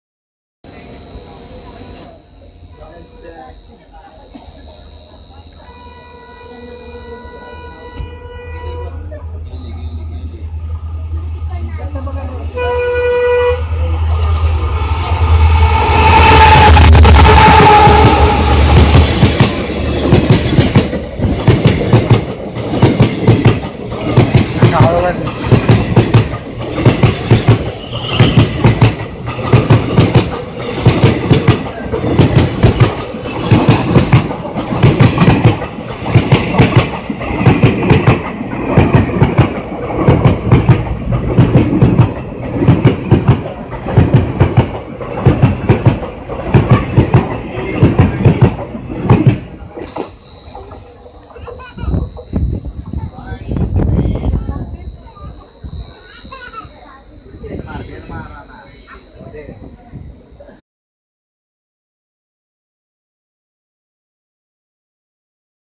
Killer Night Speed Indian Railway With Honk
Indian Railways Crossing Another Train At Konkan Railway Route with honking full speed